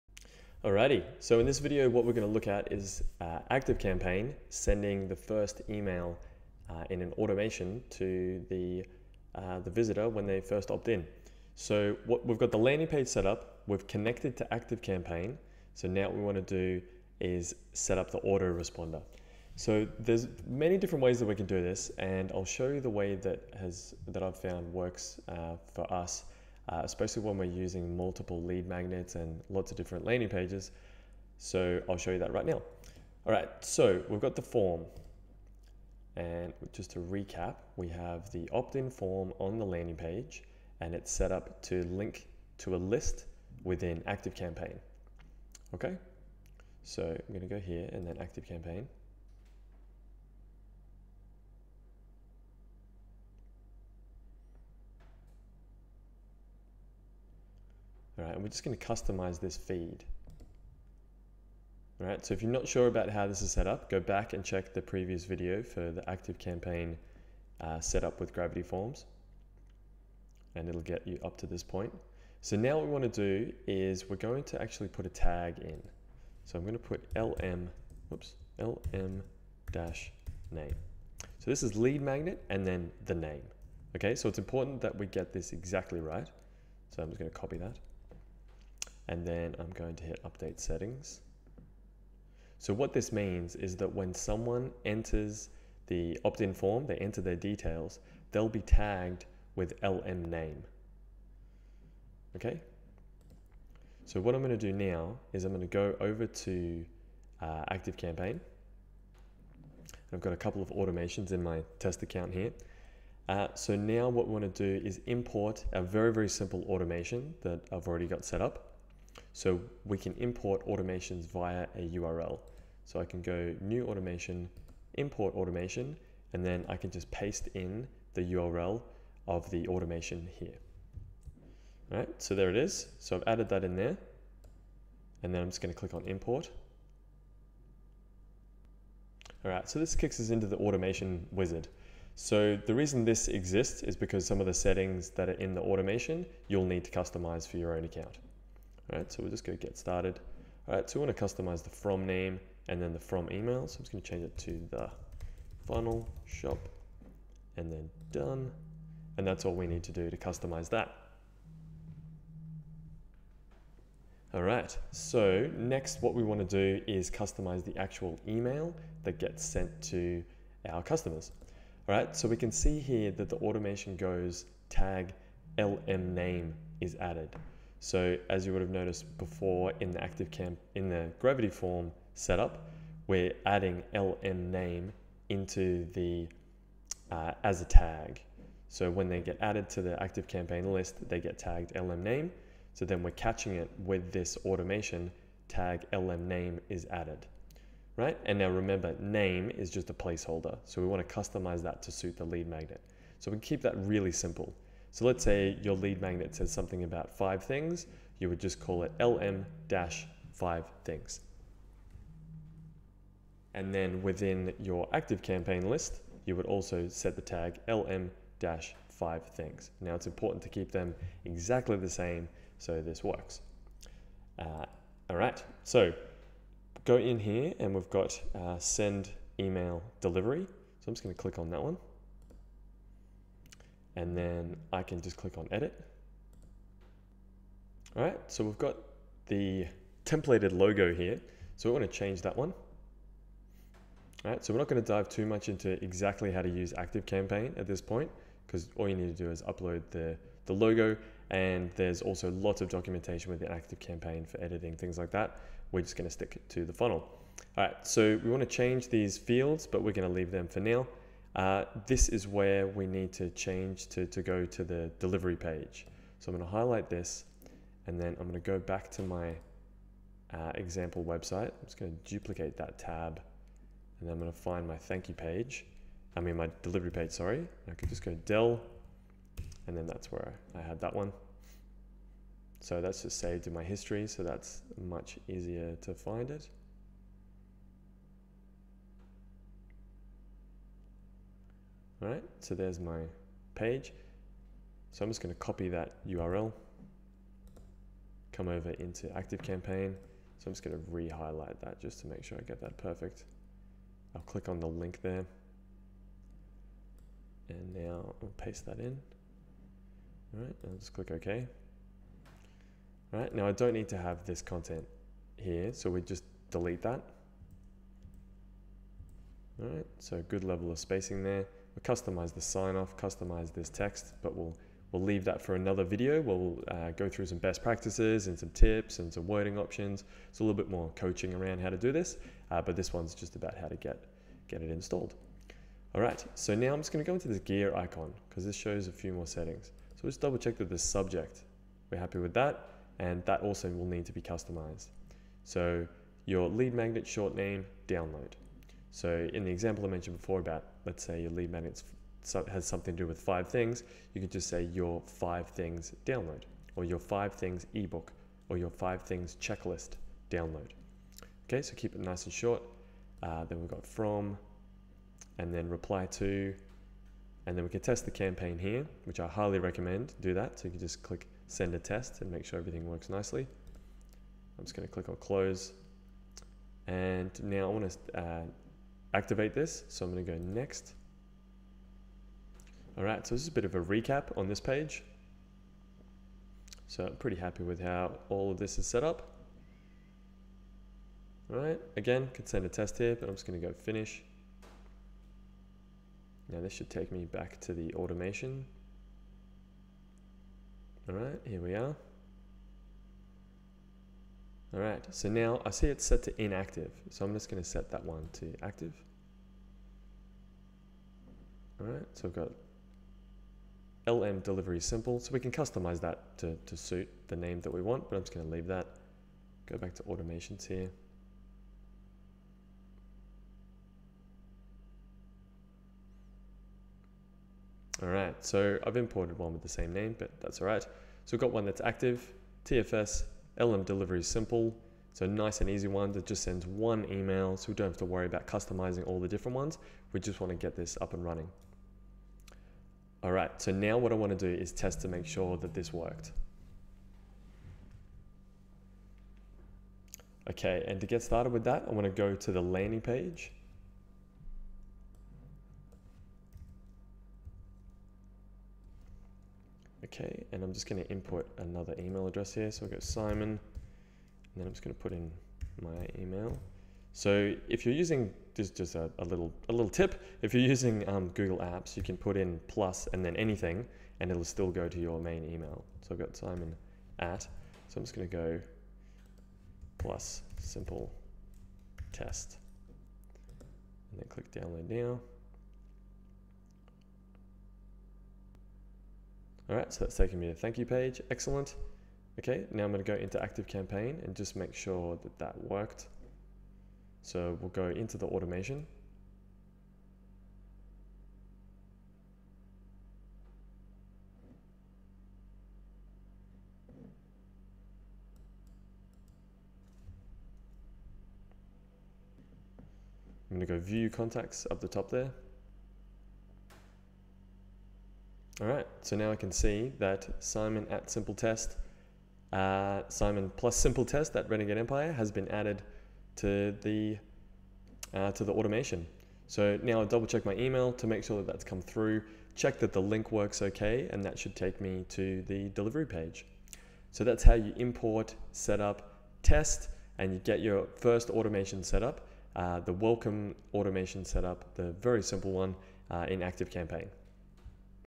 The Funnel Shop Installation Instructions - Gravity Forms Active Campaign Simple Automation
Installation instructions for your WordPress website marketing Funnel from thefunnelshop.com .